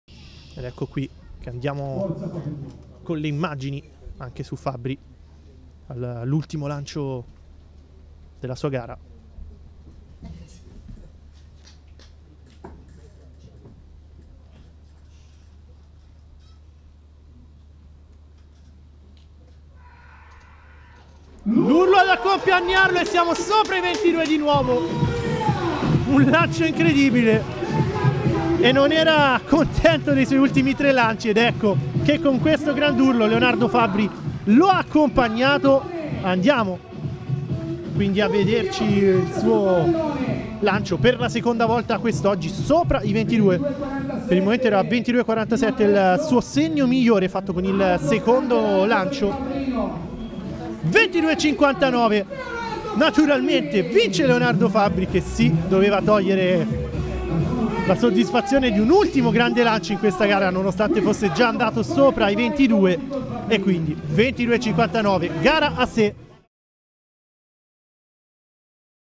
Leonardo Fabbri 22.59 nel peso al Meeting Città di Lucca
Rivedi tutto il Meeting su Sportface TV su tv.sportface.it